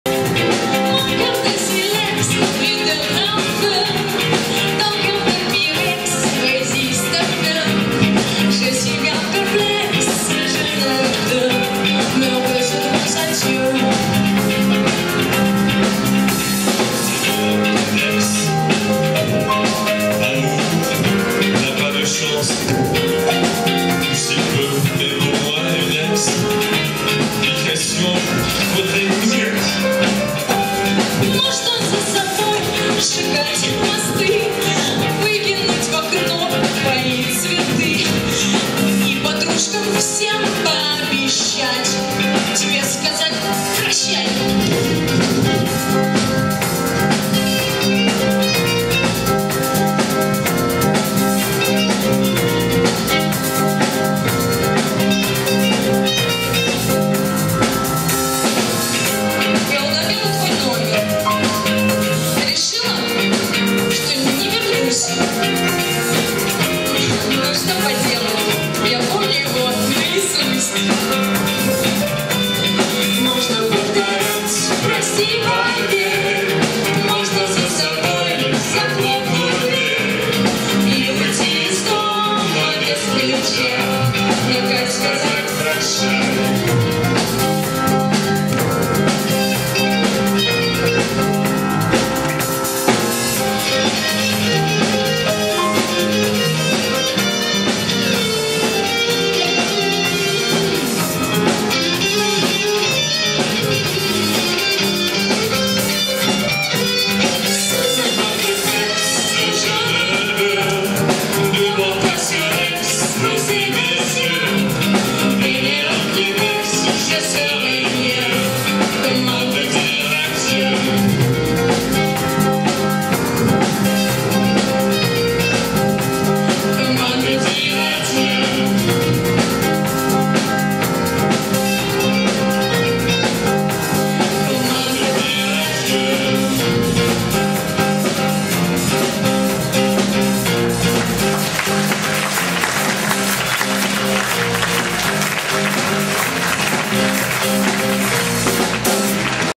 Хор Ж.Брассенса-"Serge Gainsbourg.No comment"-6
20-21 октября 2008г. в Москве в ТЦ "На Страстном" прошел фестиваль, посвященный 80-летию со дня рождения Сержа Гензбура, знаменитого шансонье и непревзойденного мастера эпатажа.